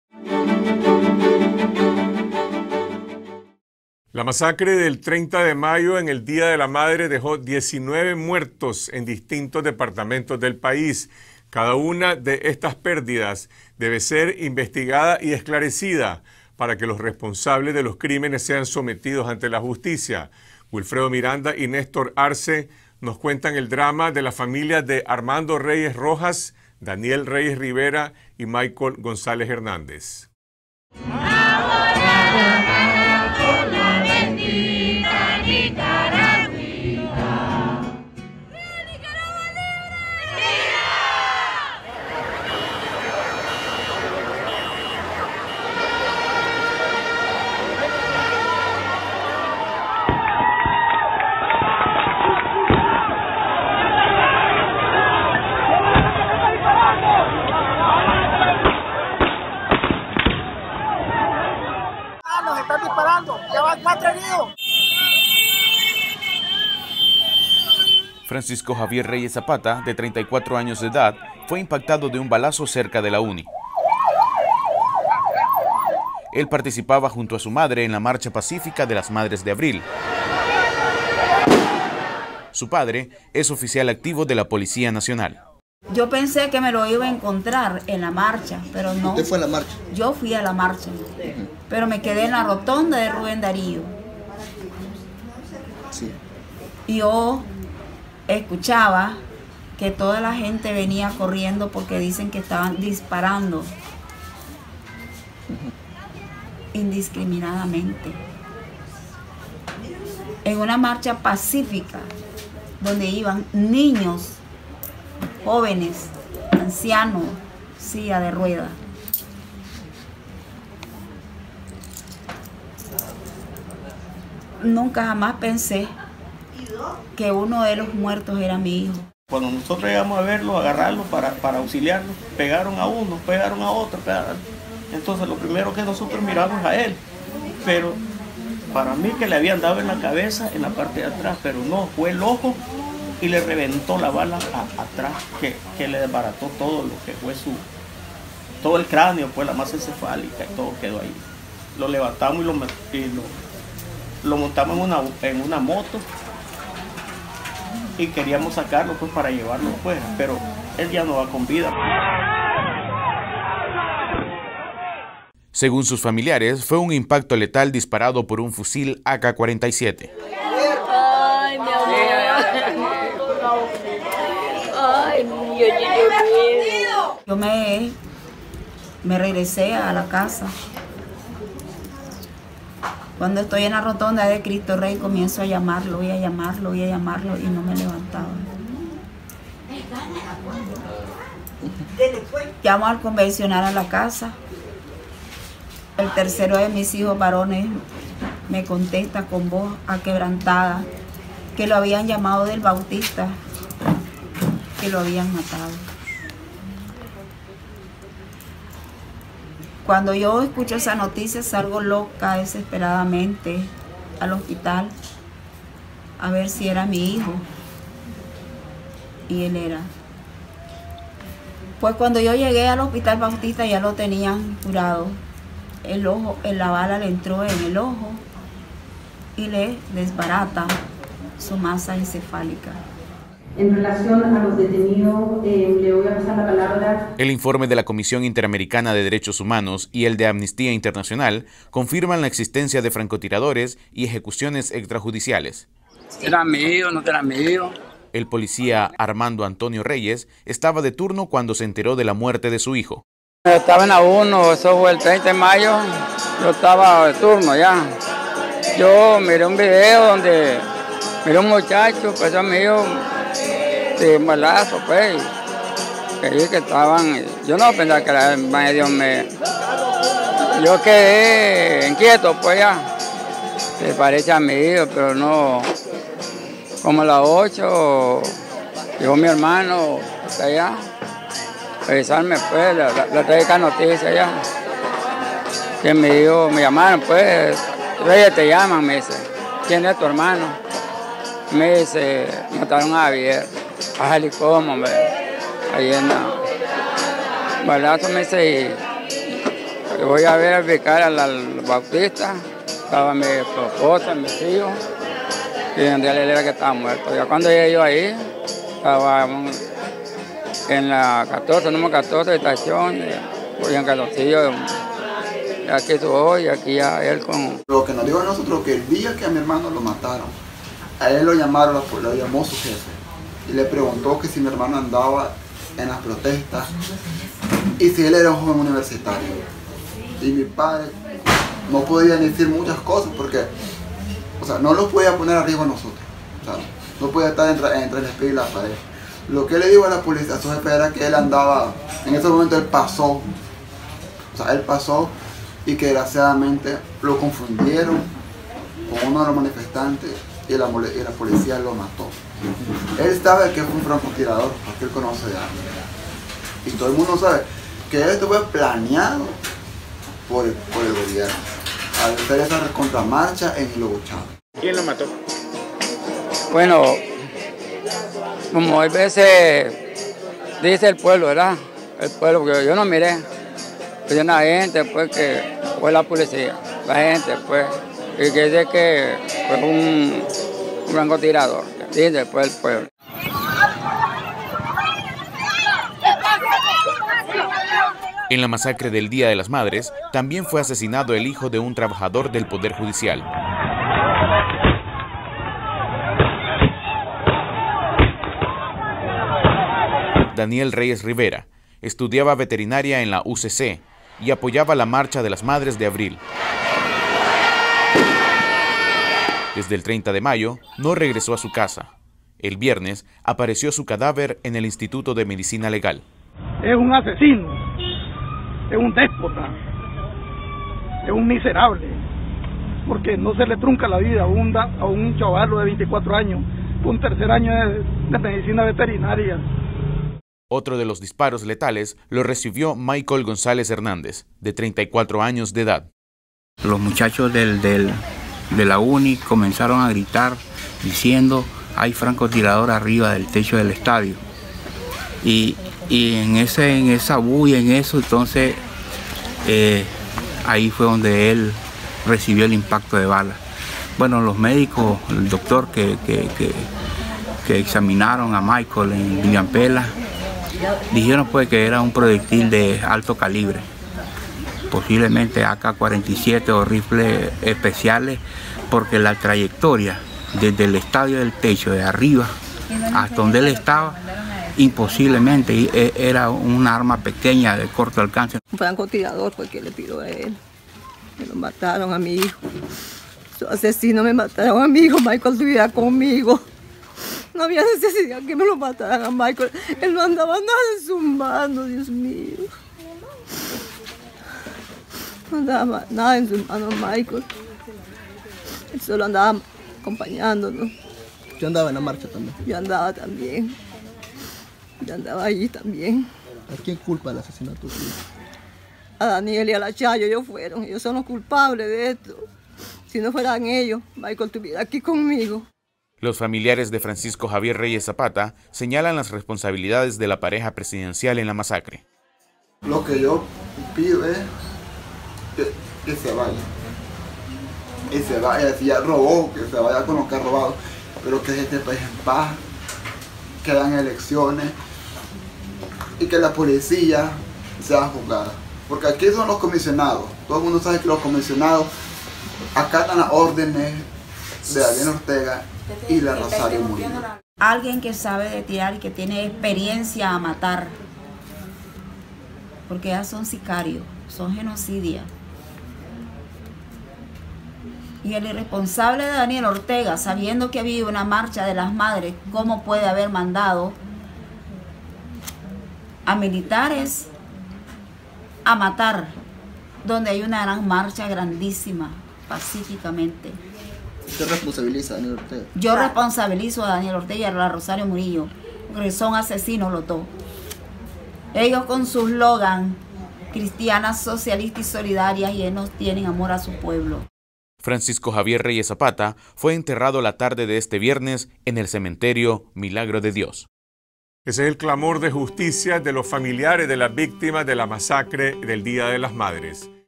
Las familias de las víctimas de la masacre del Día de las Madres: "son unos sicarios, genocidas"
Entre los muertos de la masacre del 30 de mayo, hay hijos de policías y trabajadores del estado. Las familias de las víctimas de la masacre del Día de las Madres llaman al gobierno y las fuerzas de represión "sicarios" y "genocidas".